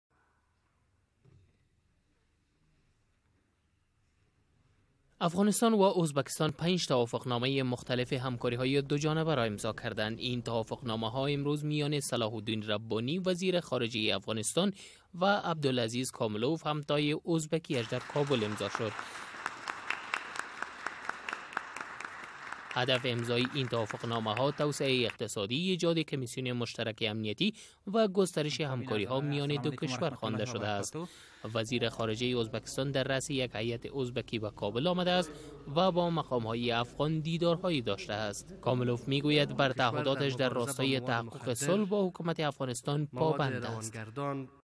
کابل و تاشکند ۵ توافق‌نامه همکاری‌های دو جانبه را امضا کردند
افغانستان و ازبکستان پنج توافق‌نامه همکاری‌های دو جانبه را امضا کردند.
این توافق‌نامه‌ها، امروز میان صلاح الدین ربانی وزیر خارجه افغانستان و عبدالعزیز کاملوف همتای ازبکستانی‌اش در کابل امضا شد.
وزیر خارجه ازبکستان در رأس یک هیئت به کابل آمده و با مقام‌های افغان دیدار های داشته‌است.